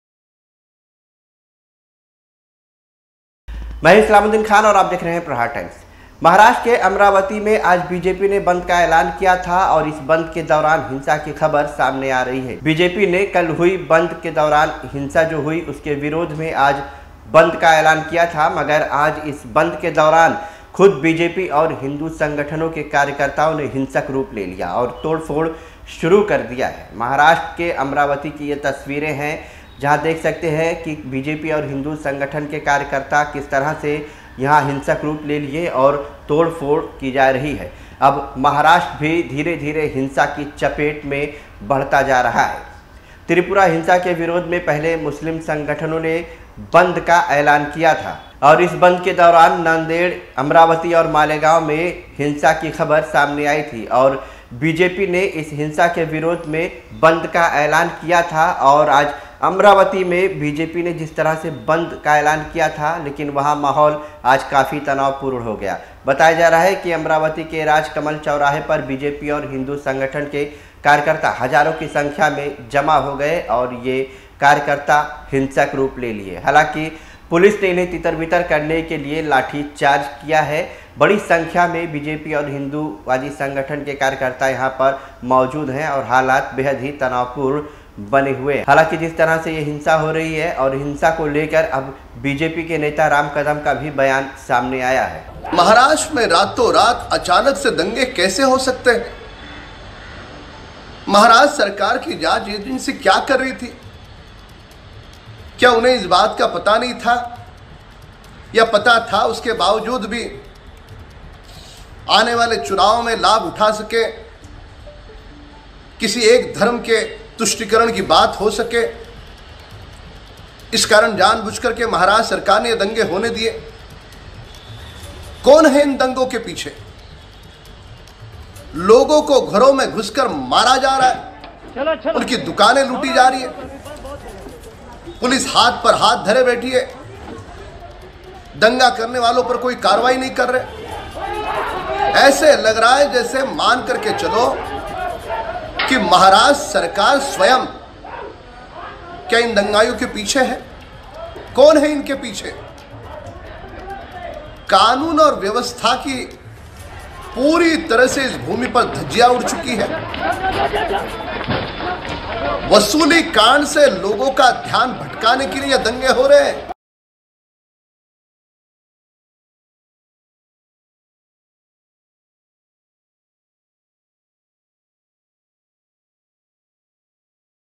अमरावती में BJP और हिंदू संगठनों ने की तोड़फोड़,पुलिस ने लाठीचार्ज किया || Islamuddin Khan
अमरावती में आज फिर BJP बंद के दौरान हिंसा भड़क गई कई जगहों पर तोडफ़ोड़ किया गया,BJP और हिन्दू संगठनों के लोग हज़ारों की संख्या में शामिल है